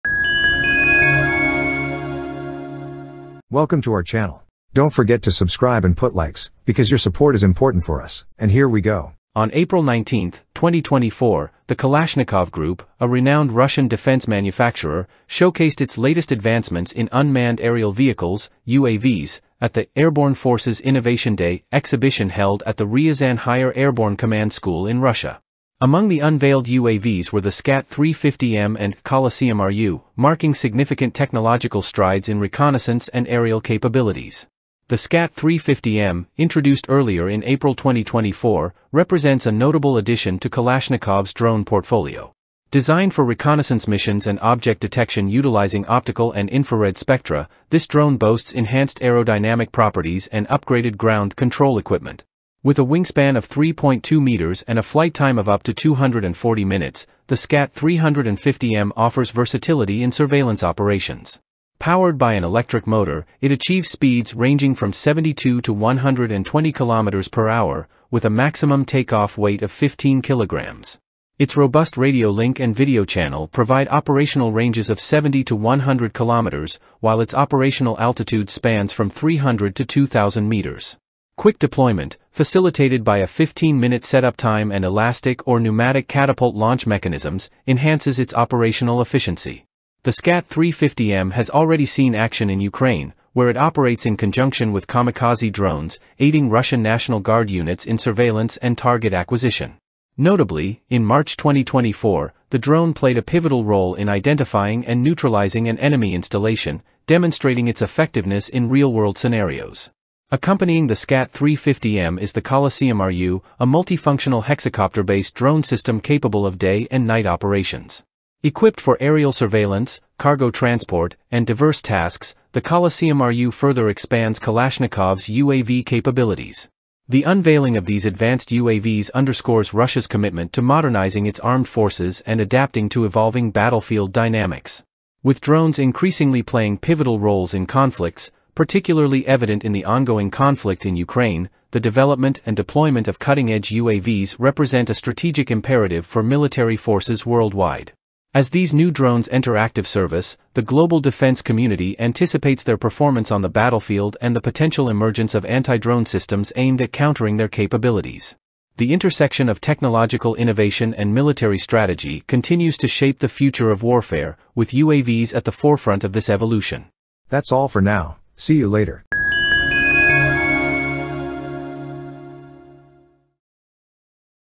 Kalashnikov Introduces SCAT 350 M and Coliseum RU Drones in Russia
On April 19, 2024, the Kalashnikov Group, a renowned Russian defense manufacturer, showcased its latest advancements in unmanned aerial vehicles (UAVs) at the "Airborne Forces Innovation Day" exhibition held at the Ryazan Higher Airborne Command School in Russia. Among the unveiled UAVs were the SCAT 350 M and "Coliseum-RU," marking significant technological strides in reconnaissance and aerial capabilities.